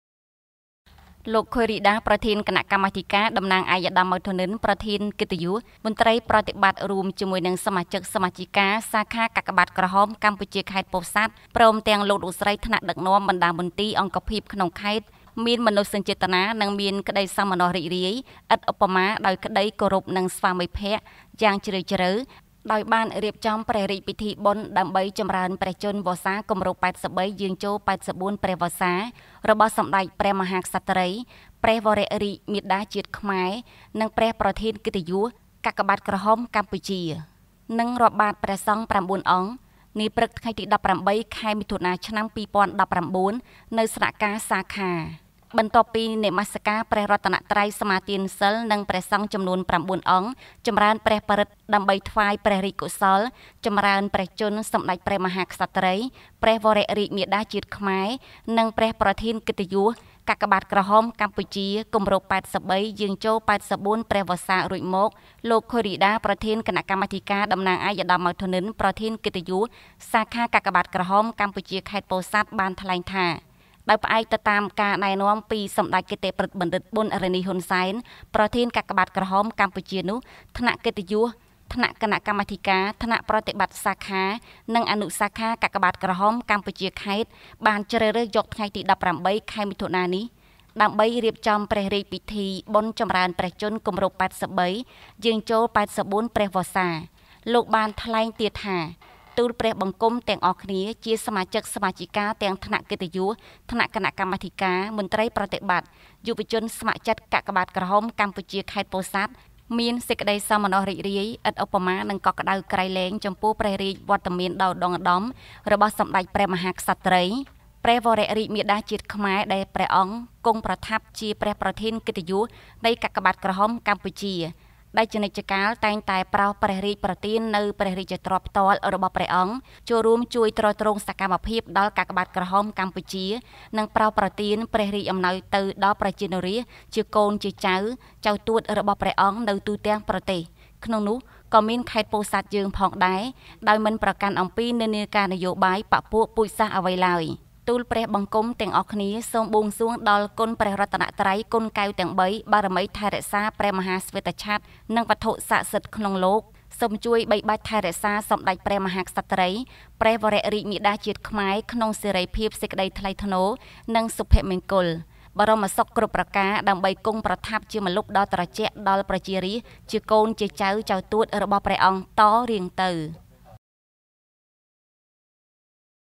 សាខាកាកបាទក្រហមកម្ពុជាខេត្តពោធិ៍សាត់ប្រារព្ធបុណ្យចម្រើនព្រះជន្មសម្តេចព្រះមហាក្សត្រី
លោកខូយ រីដា ប្រធាន គណៈកម្មាធិការ តំណាងឯកឧត្តម ម៉ៅ ធនិន ជាប្រធានកិត្តិយស មន្ត្រីប្រតិបត្តិរួមជាមួយ សមាជិកសមាជិការ សាខាកាកបាទក្រហមកម្ពុជាខេត្តពោធិ៍សាត់ ព្រមទាំងលោក លោកស្រីថ្នាក់ដឹកនាំបណ្តាមន្ទីរ អង្គភាពក្នុង ខេត្ត មានមនោសញ្ចេតនានិងមានក្តីសោមនស្សរីករាយ ឥតឧបមាដោយក្តី គោរពនិងស្វាមីភក្ត័ យ៉ាងជ្រាលជ្រៅដោយបានរៀបចំ ព្រះរាជពិធីបុណ្យ ដើម្បីចម្រើនព្រះជន្មវស្សាគម្រប់៨៣ យាង ចូល៨៤ព្រះវស្សា របស់សម្តេច ព្រះមហាក្សត្រី ព្រះវរាជមាតាជាតិខ្មែរ និងព្រះប្រធានកិត្តិយស កាកបាទក្រហមកម្ពុជា និងរាប់បាតព្រះសង្ឃ៩អង្គនាព្រឹកថ្ងៃ ទី១៨ ខែ មិថុនា ឆ្នាំ២០១៩ នៅស្នាក់ ការសាខា។
បន្ទាប់ពីនមស្សកាព្រះរតនត្រ័យ សមាតានសីលនិងព្រះសង្ឃចំនួន៩អង្គ ចម្រើនព្រះបរិត្ត ដើម្បីថ្វាយព្រះរាជកុសល ចម្រើន ព្រះជន្ម សម្តេចព្រះមហាក្សត្រី ព្រះវរាជមាតាជាតិខ្មែរ និងព្រះប្រធានកិត្តិយស កាកបាទក្រហមកម្ពុជា គម្រប់៨៣យាង ចូល ៨៤ ព្រះវស្សា រួចមក លោក ខូយ រីដា ប្រធានគណកម្មាធិការតំណាងឯកឧត្តម ម៉ៅ ធនិន ប្រធានកិត្តិយស សាខាកាកបាទក្រហមកម្ពុជា ខេត្តពោធិ៍សាត់ បានថ្លែងថា ដោយផ្អែកទៅតាម ណែនាំពីសម្តេចកិត្តិព្រឹទ្ធបណ្ឌិត ប៊ុន រ៉ានី ហ៊ុន សែន ប្រធានកាកបាទ ក្រហម កម្ពុជានោះ ថ្នាក់គណៈកិត្តិយស ថ្នាក់គណៈកម្មាធិការ  ថ្នាក់ប្រតិបត្តិសាខា និងអនុសាខាកាកបាទក្រហមកម្ពុជាខេត្ត បានជ្រើសរើស យកថ្ងៃ ទី១៨ ខែមិថុនានេះឡើងដើម្បីរៀបចំព្រះរាជ ពិធីបុណ្យចម្រើនព្រះជន្មគម្រប់៨៣ និងយាងចូល៨៤ព្រះវស្សា។ លោកបាន ថ្លែងទៀតថា ទួលព្រះបង្គំទាំងអស់គ្នា ជាសមាជិក សមាជិការទាំងថ្នាក់កិត្តិយស ថ្នាក់គណៈកម្មាធិការ មន្ត្រីប្រតិបត្តិ យុវជនស្ម័គ្រចិត្តសាខាកាកបាទក្រហមកម្ពុជា ខេត្តមានសេចក្តី សោមនស្សរីករាយឥតឧបមានិងកក់ក្តៅក្រៃលែងចំពោះព្រះរាជវត្តមានដ៏ឧត្តុង្គឧត្តម របស់សម្តេច ព្រះមហាក្សត្រី ព្រះវរាជមាតាជាតិ ខ្មែរ ដែលព្រះអង្គគង់ប្រថាប់ ជាព្រះប្រធានកិត្តិយសនៃកាកបាទក្រហម កម្ពុជា ដែលជា និច្ចកាលតែងតែប្រោសព្រះរាជប្រទាននូវ ព្រះរាជទ្រព្យផ្ទាល់ របស់ព្រះអង្គចូលរួមជួយទ្រទ្រង់សកម្មភាព ដល់កាក បាទក្រហមកម្ពុជានិងប្រោសប្រទាន ព្រះរាជអំណោយទៅ ដល់ប្រជានុរាស្ត្រ ជាកូន ជាចៅ ចៅទួតរបស់ព្រះអង្គនៅទូទាំងប្រទេសក្នុងនោះ ក៏មានខេត្តពោធិ៍សាត់យើង ផងដែរ ដោយមិនប្រកាន់ អំពីនិន្នាការនយោបាយ បក្ខពួក ពួជសាសន៍អ្វីឡើយ។ ទួលព្រះបង្គំ ទាំងអស់គ្នាសូមបួងសួងដល់គុណព្រះរតនត្រ័យគុណកែវទាំងបី បារមីថែរក្សាព្រះមហាស្វេតឆត្រ័និងវត្ថុសក្តិសិទ្ធិក្នុងលោក សូមជួយ បីបាច់ថែរក្សាសម្តេចព្រះមហាក្សត្រីព្រះវរាជមាតាជាតិខ្មែរ ក្នុងសេរី សេចក្តីថ្លៃថ្នូនិងសុភមង្គល បរមសុខគ្រប់ប្រការ ដើម្បីគង់ប្រថាប់ ជាម្លប់ដ៏ត្រជាក់ដល់ប្រជារាស្ត្រ ជាកូន ជាចៅ ចៅទួត របស់ ព្រះអង្គតរៀងទៅ៕ ស្ងួន ឈឿន ពោធិ៍សាត់